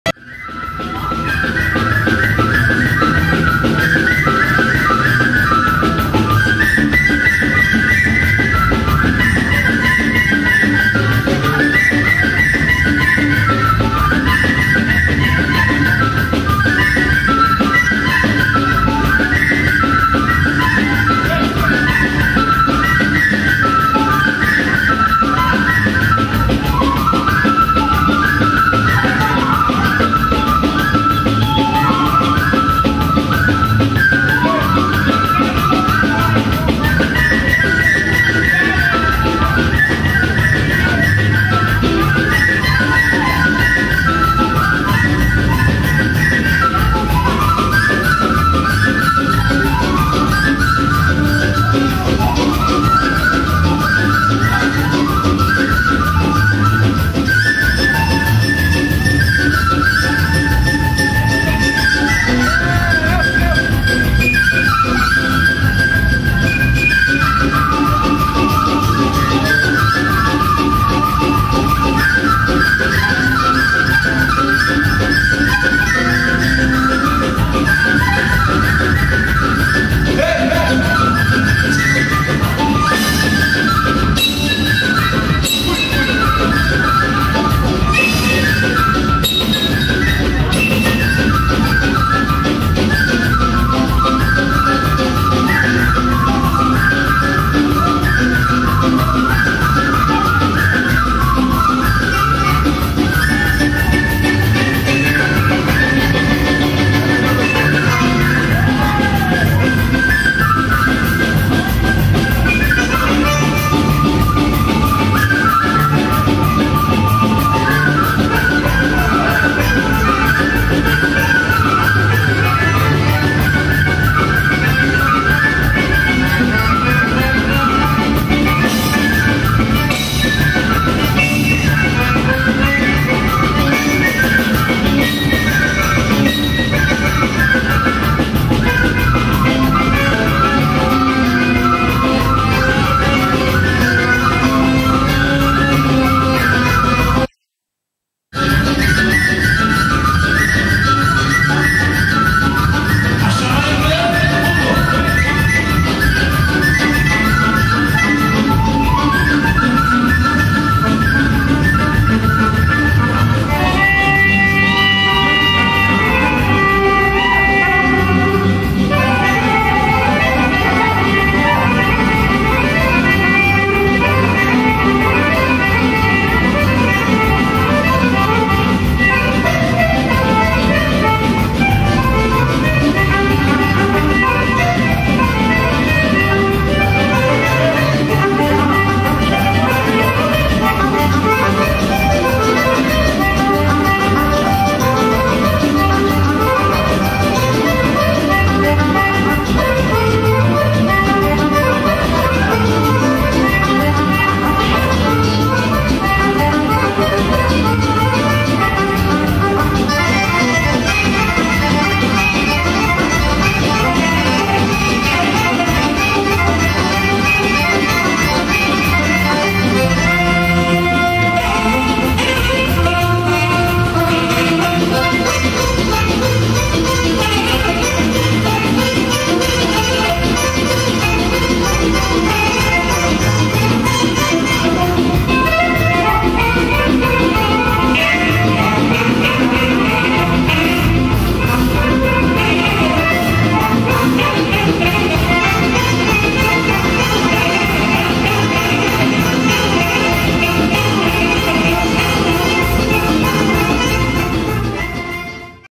Vlasko Kolo FRULA
Veridba- Danijele Ilic i Slavomira Grujica u Becu
Linzerstrasse. Ljubica Boldeskic  i Branko i Ljubisa Bozinovic na Harmonici i ostali virtuozi.